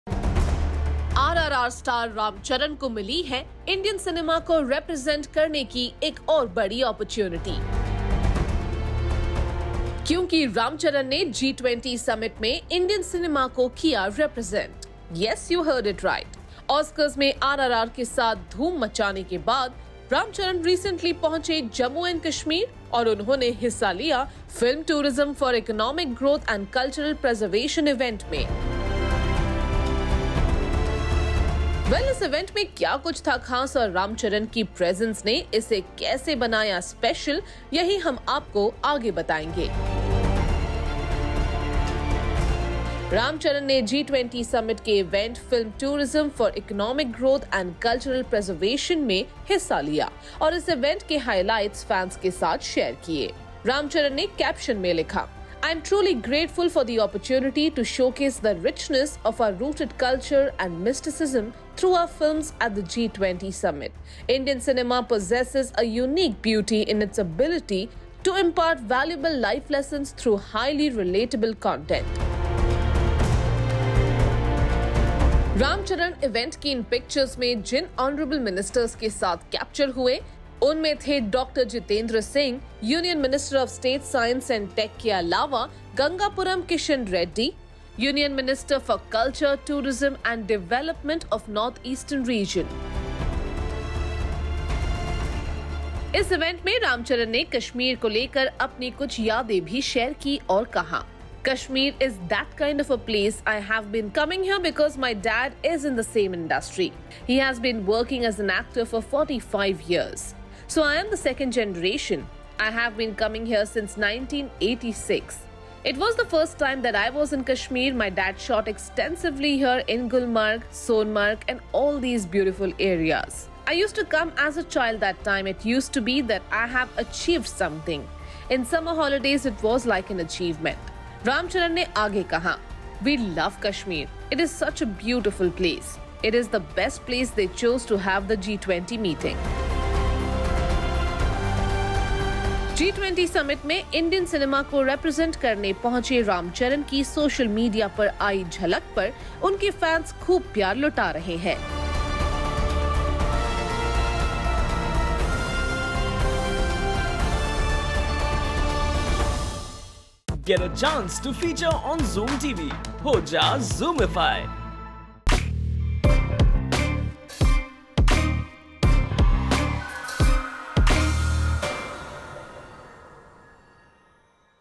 Ram Charan represents Indian cinema at G20 summit; pens an emotional note | Bollywood News
Bollywood News - Ram Charan took to his social media and shared some pictures from recent G20 summit that was held in Jammu and Kashmir. The actor also expressed his gratitude for the opportunity. Listen in!